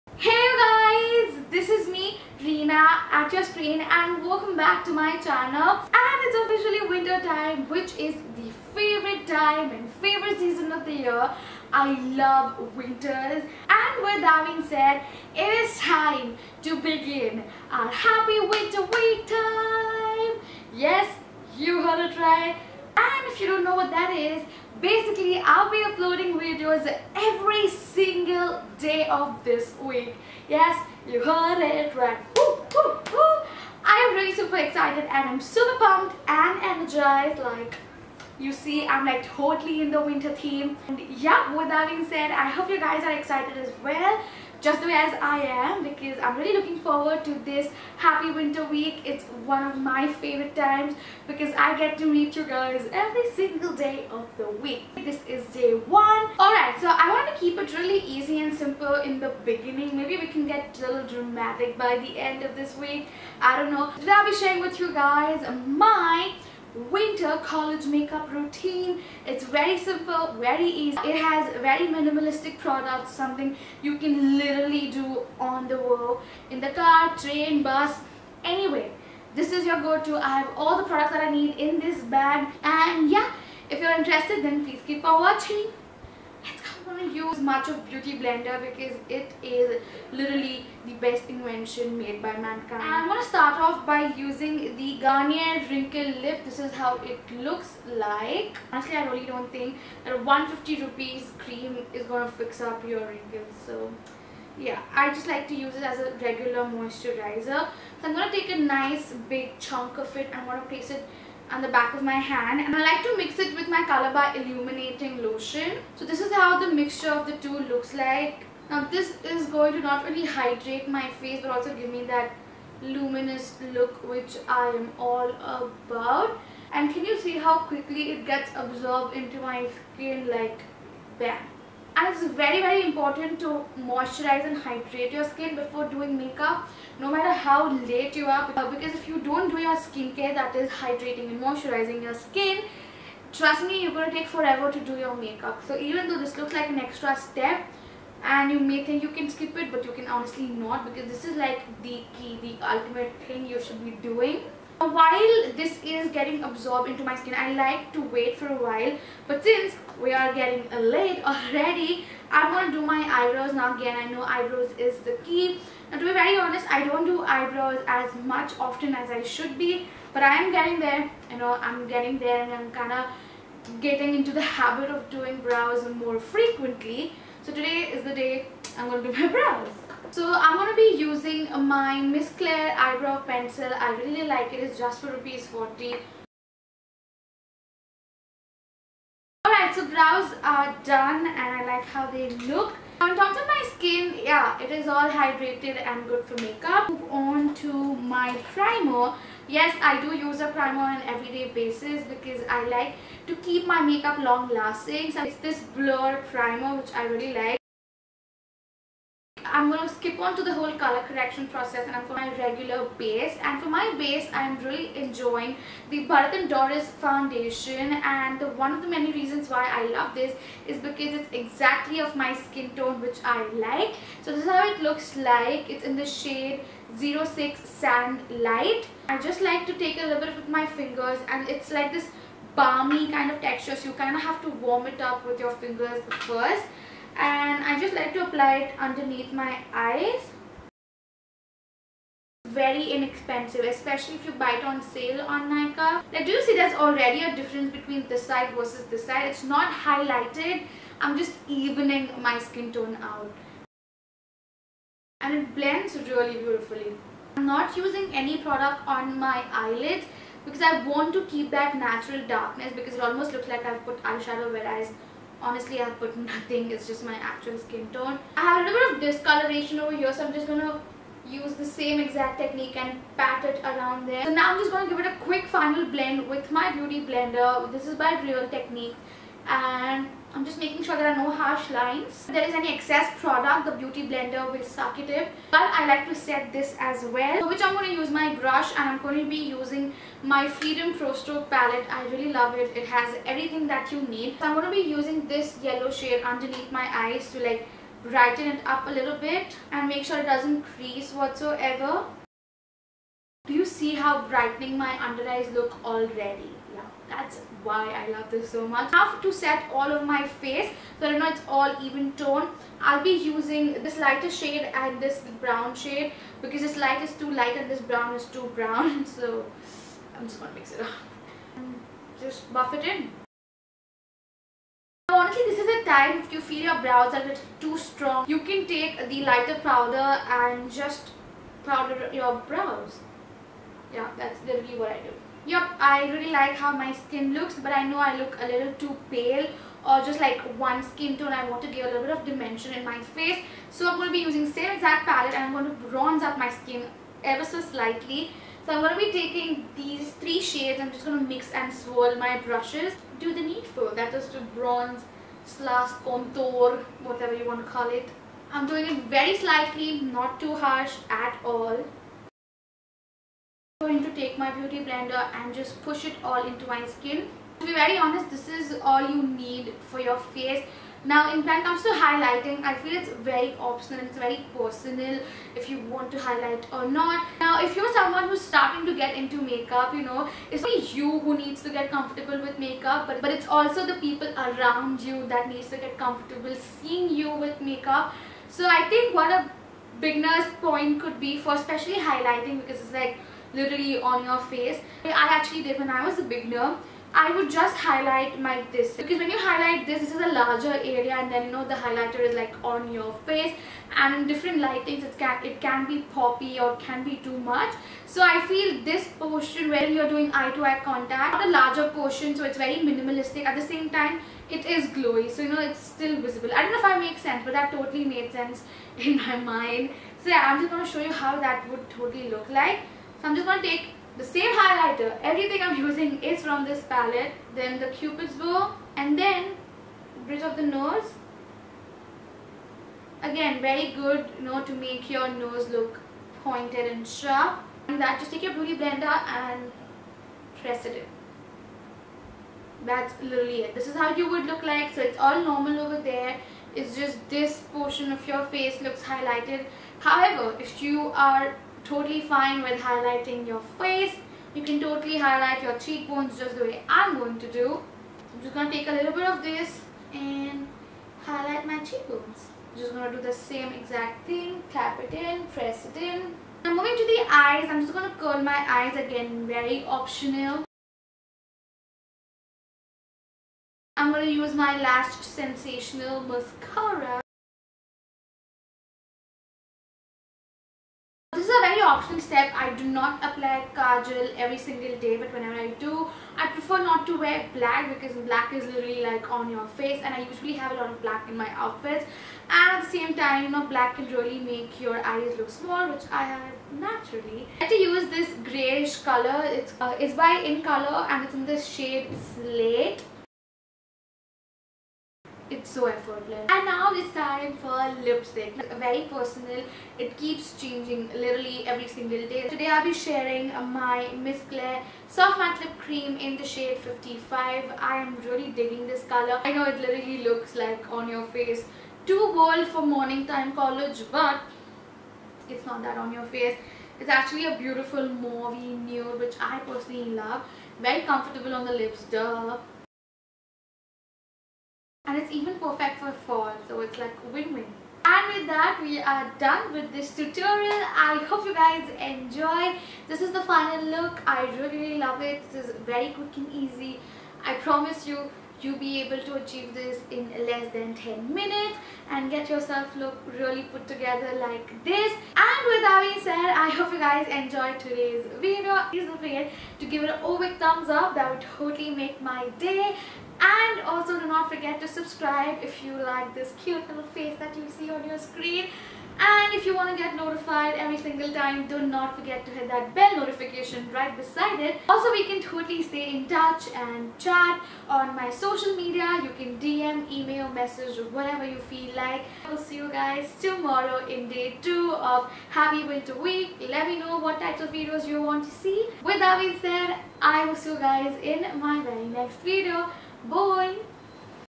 My Everyday Makeup Routine for College 2017| Happy winter week | DAY 1
I AM SOO FREAKING EXCITED FOR THIS WEEK . I WILL BE UPLOADING EVERY SINGLE DAY OF THIS WEEK. I HOPE YOU ARE AS EXCITED AS I AM XOXO LOVE

PRODUCTS USED
Garnier winkle lift cream
Colorbar illuminating lotion
Miss Claire eebrow pencil - brown
lakme blur perfect primer
Bharat and Dorais - 06 sand light
Freedom pro strobe palette
Maybelline lash sensational mascara
Incolor gel pencil liner - Slate
Miss Claire lip cream - 55